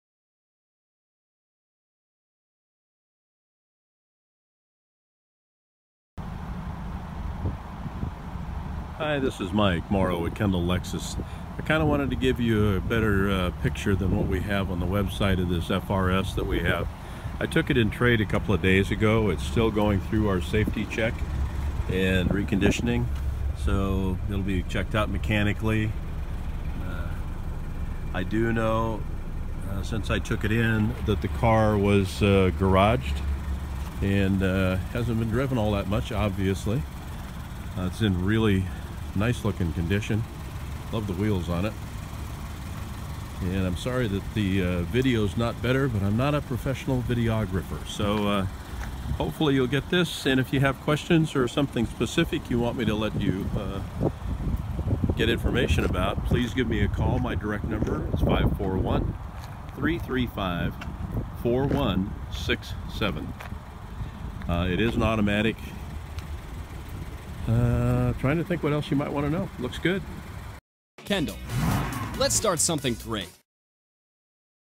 2013 Scion FR-S
Walk around of the 2013 Scion FR-S

Visit us today at:
344 Goodpasture Island Rd Eugene, OR 97401